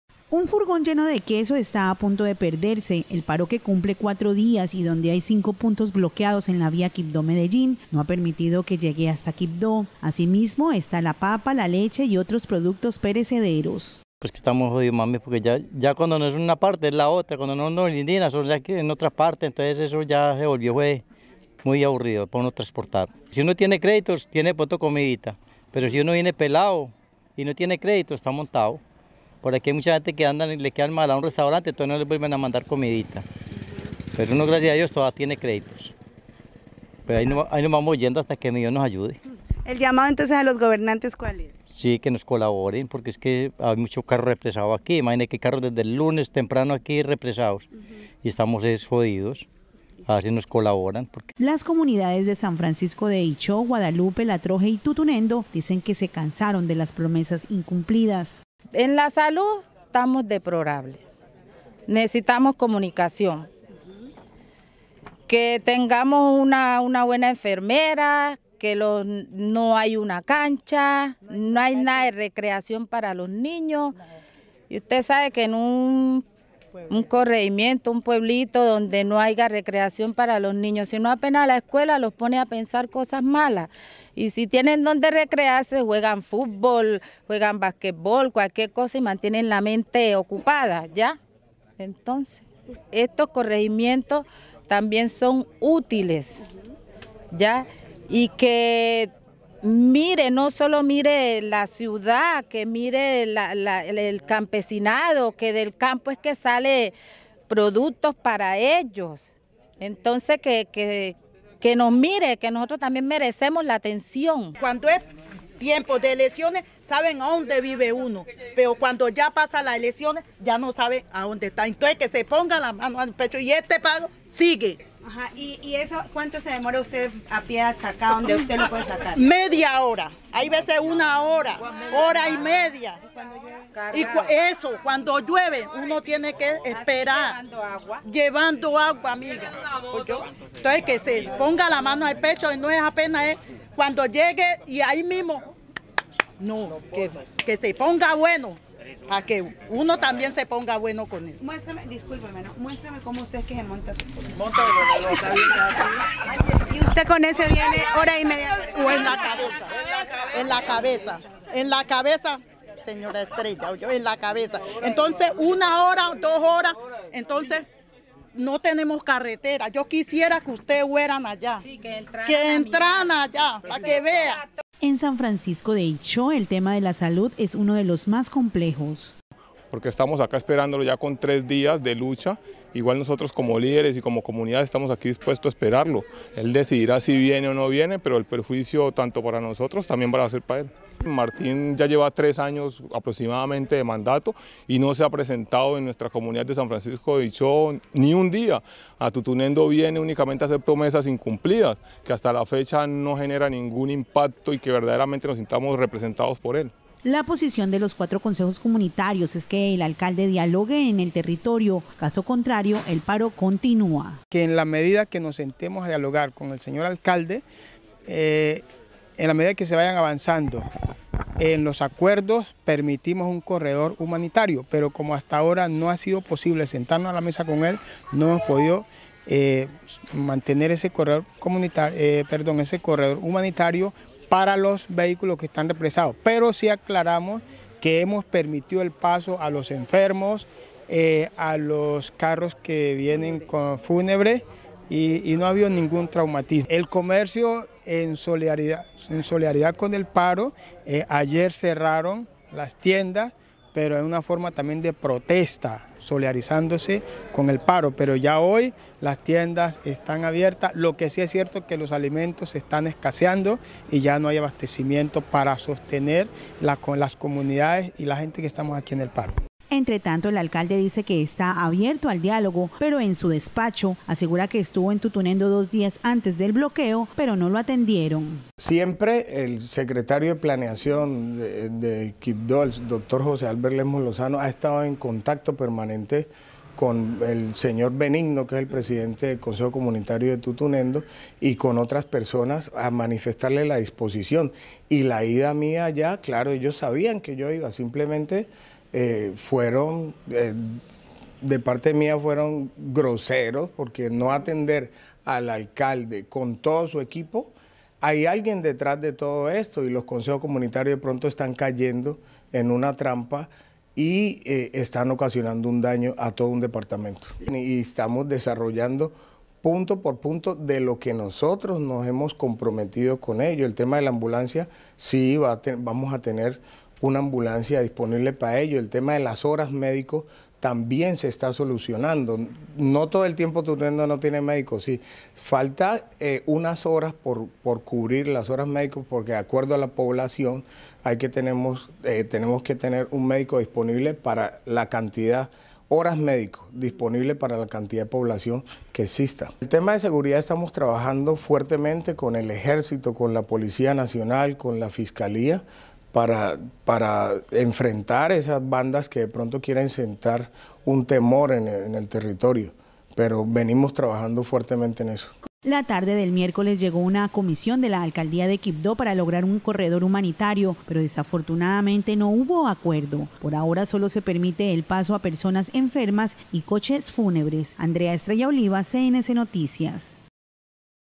"Si el alcalde no viene, el paro sigue": Líderes del Paro
Tras cuatro días de bloqueo en la vía Quibdó - Medellín  en cinco diferentes puntos, los transportadores de carga y pasajeros están desesperados, incluso los alimentos perecederos ya se están dañando, los líderes de la protesta insisten en la presencia del alcalde de Quibdó para dar vía libre.